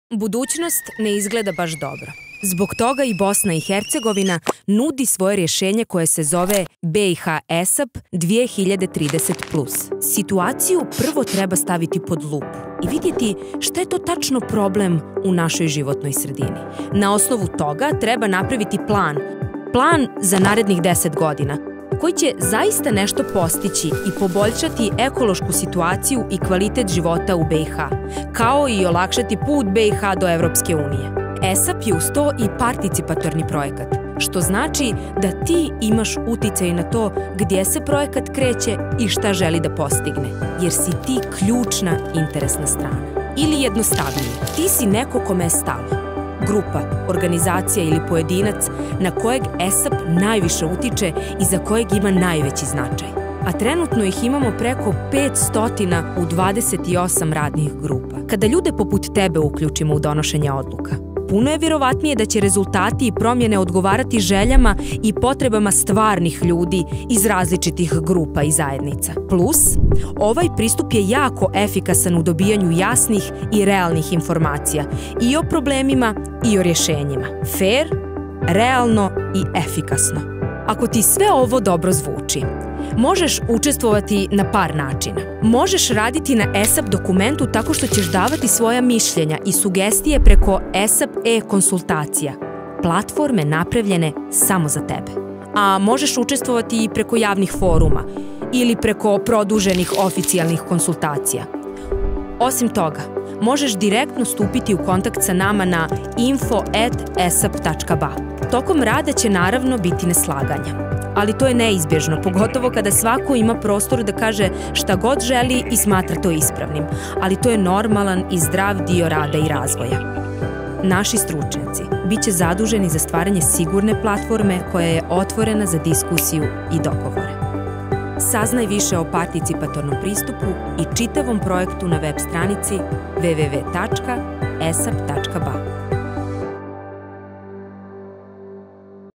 Explainer video on participatory approaches in the BiH ESAP project (subtitles in English
To ensure the ESAP development process is participatory and inclusive we invite stakeholders to provide expertise and knowledge about environmental problems and their solutions.
Watch this video which in simple terms describe the usefulness of a participatory approach and how you can engage in the ESAP project.